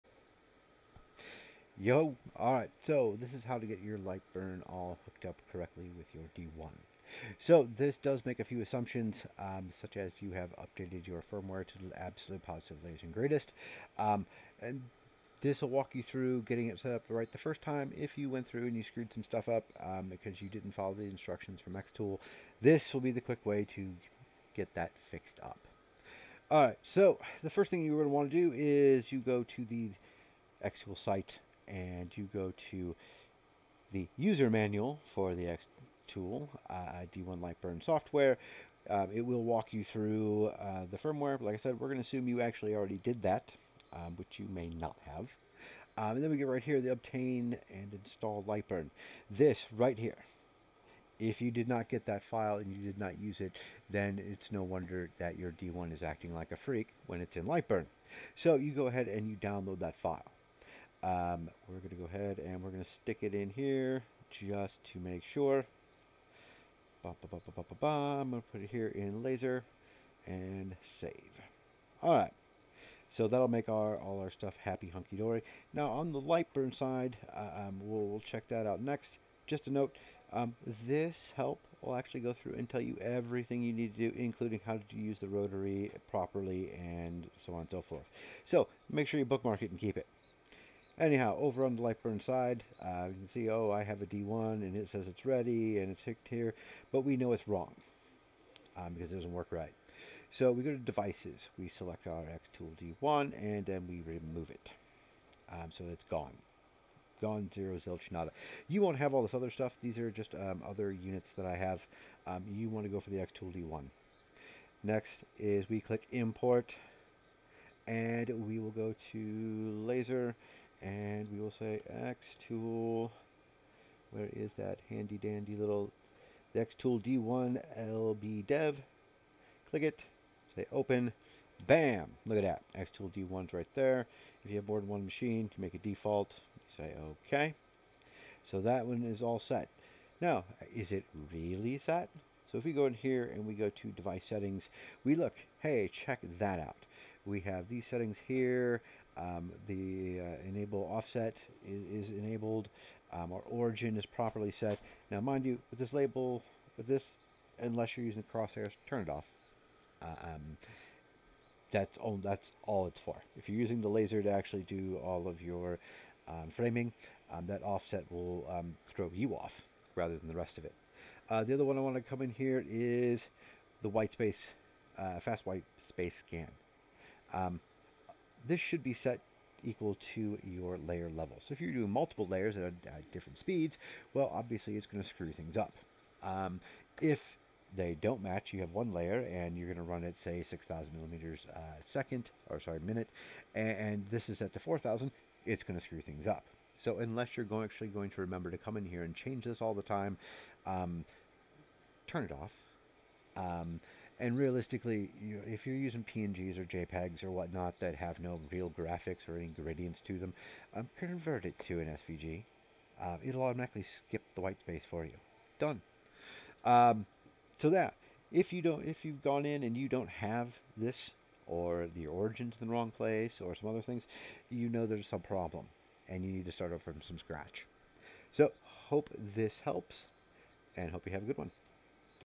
Correctly setting up the xTool D1 with Lightburn
There is a rash of issues happening due to new owners winging it with Lightburn setup instead of following xTool's instructions. Engravings are upside down, movement is jerky, etc. This video goes over for to fix that.